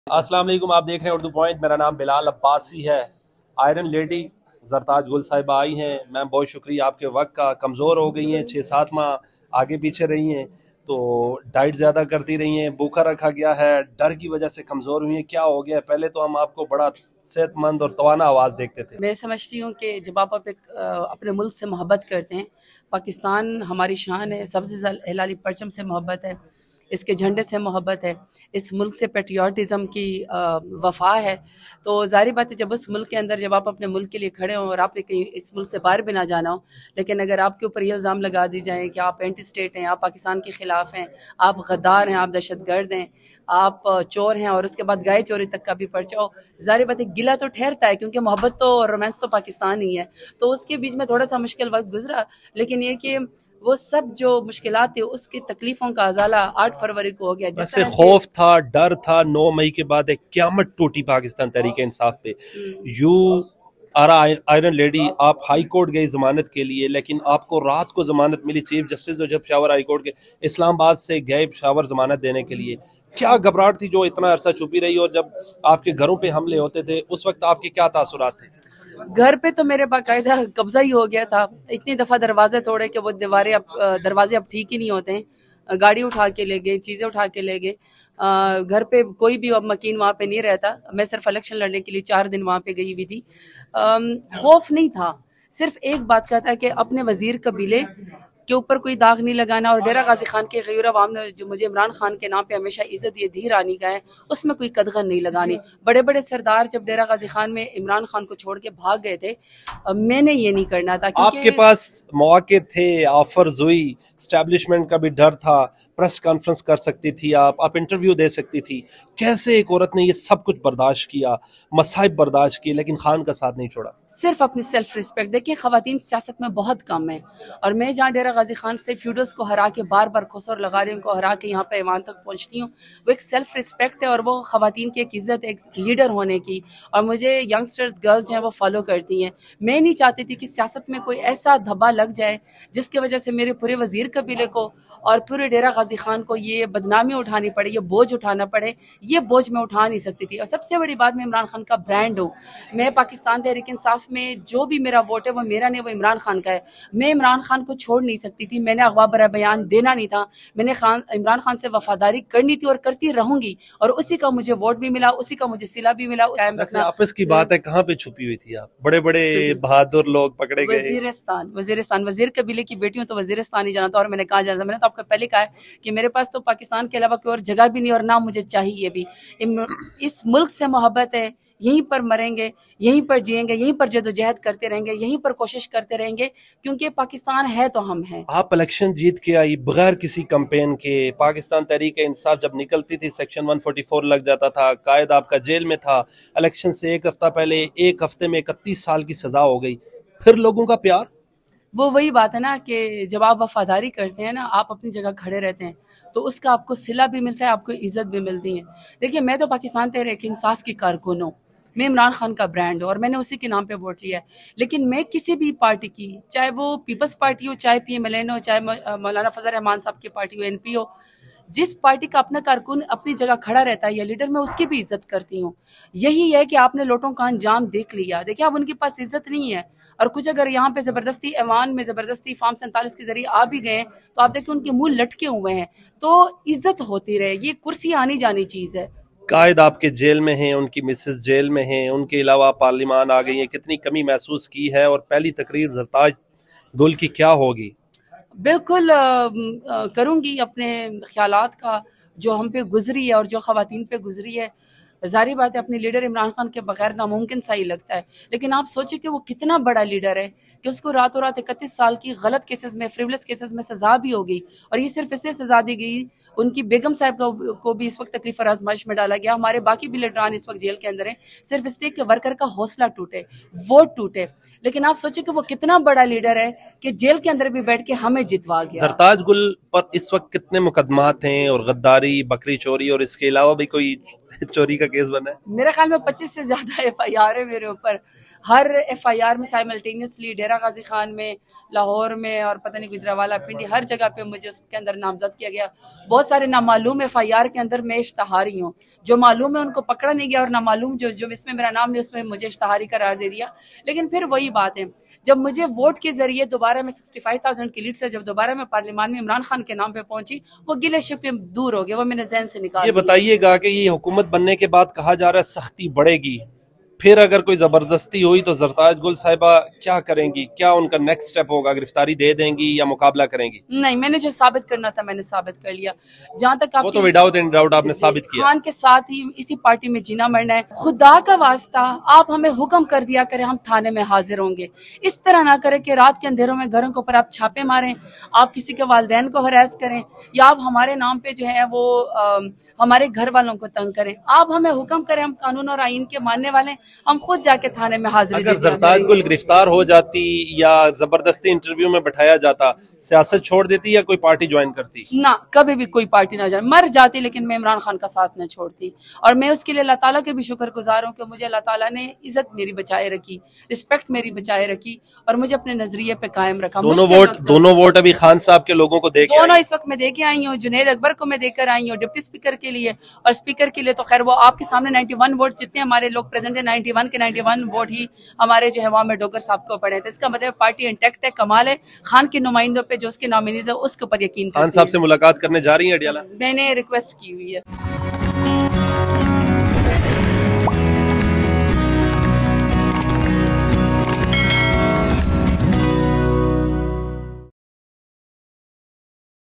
PTI Leader Zartaj Gul Interview - Mein Imran Khan Ka Brand Hoon - Police Ne Ishtihari Qarar De Diya
“Unveiling Secrets: Zartaj Gul’s Shocking Revelations in Exclusive Interview”
In this gripping interview, we delve into the mysterious past of Zartaj Gul. The police have taken notice, and the intrigue is palpable. What was concealed? Join us as we uncover the truth behind her enigmatic journey. From candid confessions to behind-the-scenes revelations, this video promises an eye-opening experience.
Anchor: Bilal Abbasi